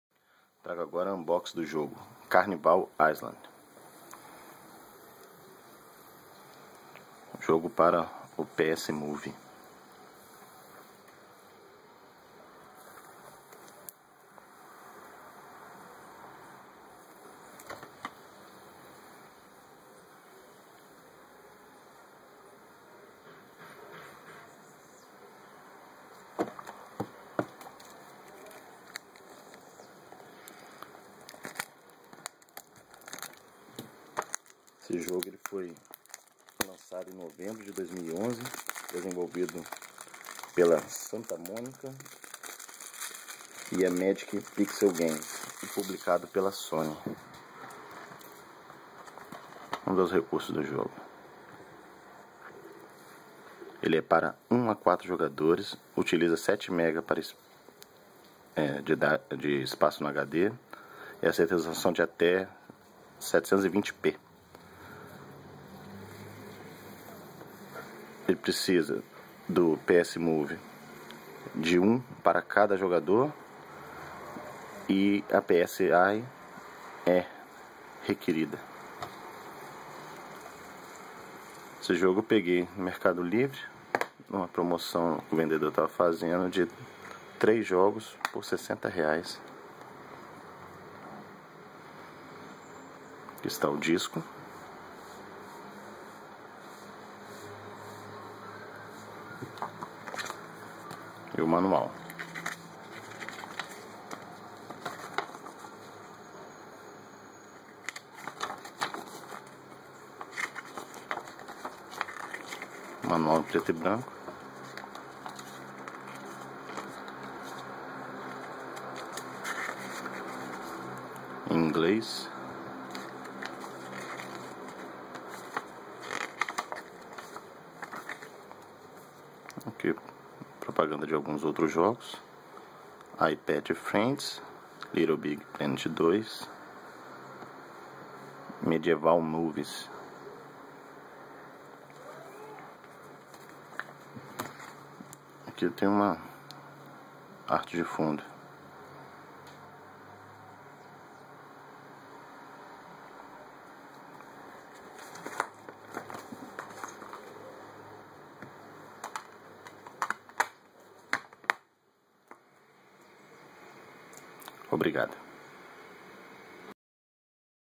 [Unboxing - PS3] Carnival Island - PT-BR
Unboxing em português do jogo Carnival Island para Playstation 3.